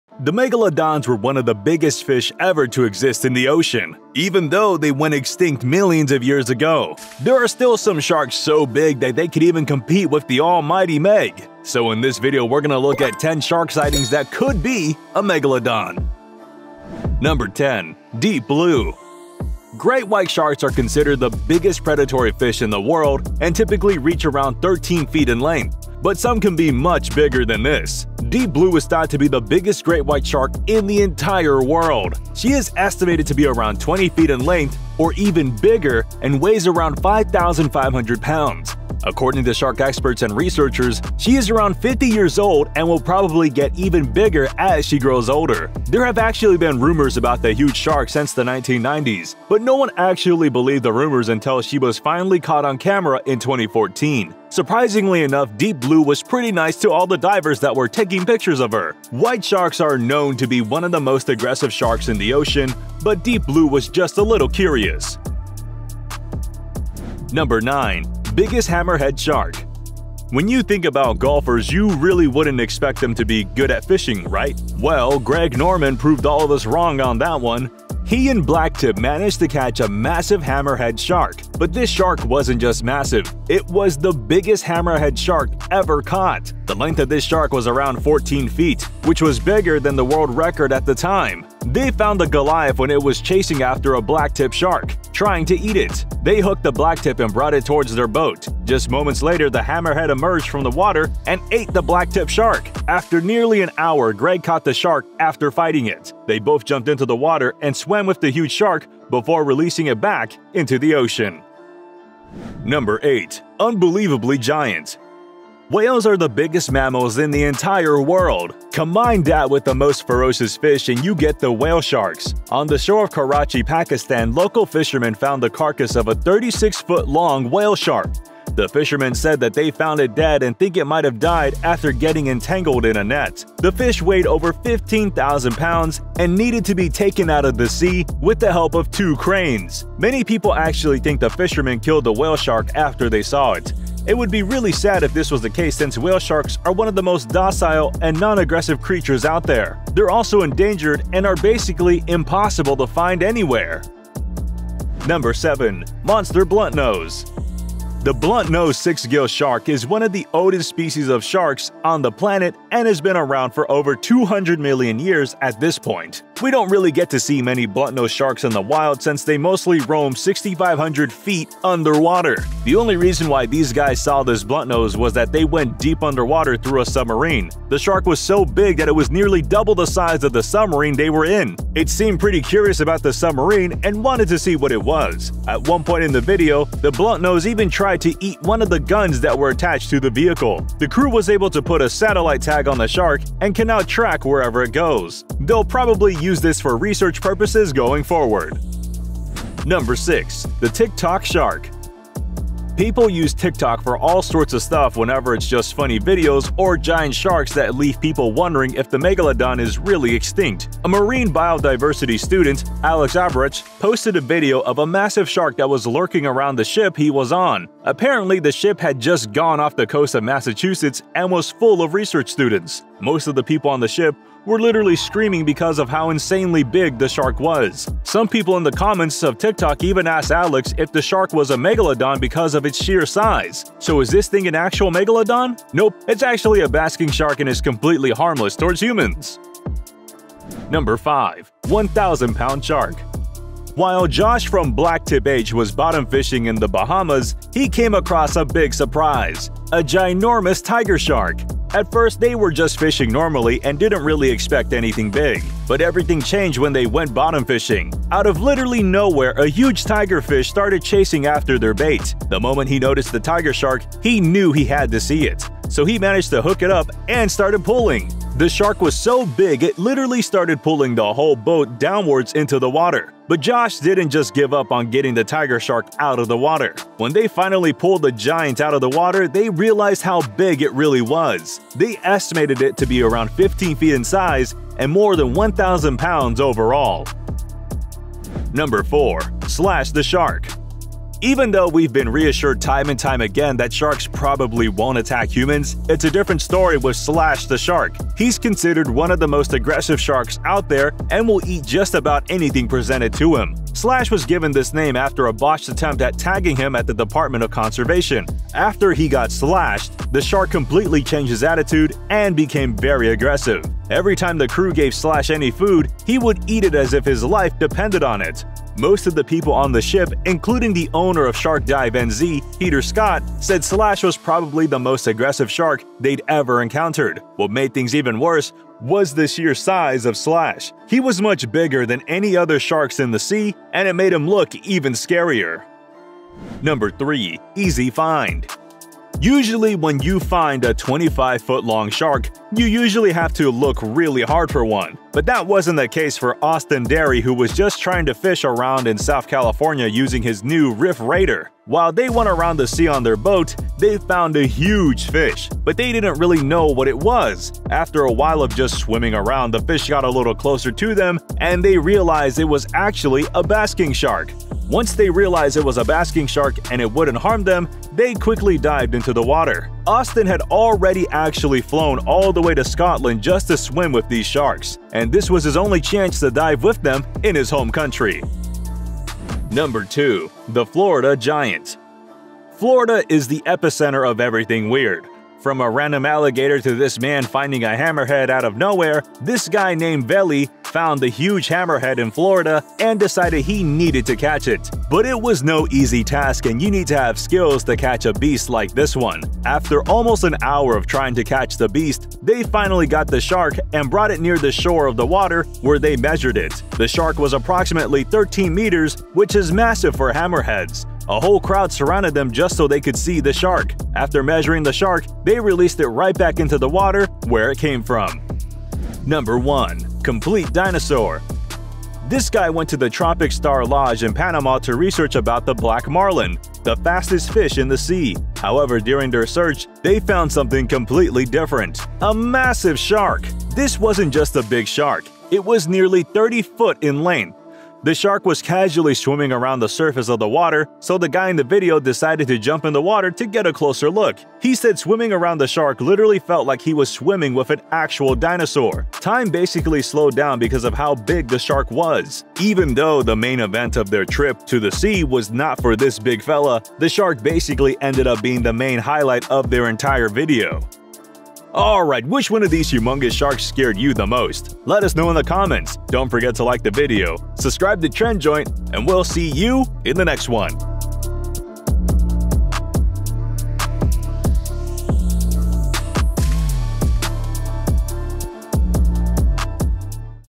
10 Shark Sightings That Could Be Megalodon
The megalodons were one of the biggest fish ever to exist in the ocean! Even though they went extinct millions of years ago, there are still some sharks so big that they could even compete with the al-mighty meg. So, in this video, we’re gonna look at 10 shark sightings that could be a megalodon!

10. Deep Blue
Great white sharks are considered the biggest predatory fish in the world and typically reach around 13-feet in length. But some can be much bigger than this! Deep blue is thought to be the biggest great white shark in the entire world! She is estimated to be around 20-feet in length or even bigger and weighs around 5,500 pounds. According to shark experts and researchers, she is around 50-years-old and will probably get even bigger as she grows older.

There have actually been rumors about the huge shark since the 1990s, but no one actually believed the rumors until she was finally caught on camera in 2014. Surprisingly enough, Deep Blue was pretty nice to all the divers that were taking pictures of her. White sharks are known to be one of the most aggressive sharks in the ocean, but Deep blue was just a little curious!

9. Biggest Hammerhead shark
When you think about golfers, you wouldn’t really expect them to be good at fishing right? Well, Greg Norman proved all of us wrong on that one! He and BlacktipH managed to catch a massive hammerhead shark. But this shark wasn’t just massive, it was the biggest hammerhead shark ever caught! The length of this shark was around 14 feet, which was bigger than the world record at the time.

They found the goliath when it was chasing after a Blacktip shark, trying to eat it. They hooked the Blacktip and brought it towards their boat. Just moments later, the hammerhead emerged from the water and ate the Blacktip shark! After nearly an hour, Greg caught the shark after fighting it. They both jumped in the water and swam with the huge shark before releasing it back into the ocean!

8. Unbelievably giant
Whales are the biggest mammals in the entire world, combine that with the most ferocious fish and you get the whale sharks! On the shore of Karachi, Pakistan, local fishermen found the carcass of a 36-foot-long whale shark! The fishermen said they found it dead and think it might have died after getting entangled in a net.

The fish weighed over 15,000 pounds and needed to be taken out of the sea with the help of two cranes! Many people actually think the fishermen killed the whale shark after they saw it. It would be really sad if that was the case since whale sharks are one of the most docile and non-aggressive creatures out there. They’re also endangered and are basically impossible to find anywhere!

7. Monster Bluntnose
The bluntnose sixgill shark is one of the oldest species of sharks on the planet and has been around for over 200 million years at this point. We don’t really get to see many bluntnose sharks in the wild since they mostly roam 6,500 feet underwater! The only reason why these guys saw this bluntnose was that they went deep underwater through a submarine.

The shark was so big that it was nearly double the size of the submarine they were in! It seemed pretty curious about the submarine and wanted to see what it was. At one point in the video, the bluntnose even tried to eat one of the guns that were attached to the vehicle.

The crew was able to put a satellite tag on the shark and can now track wherever it goes. They’ll probably use this for research purposes going forward!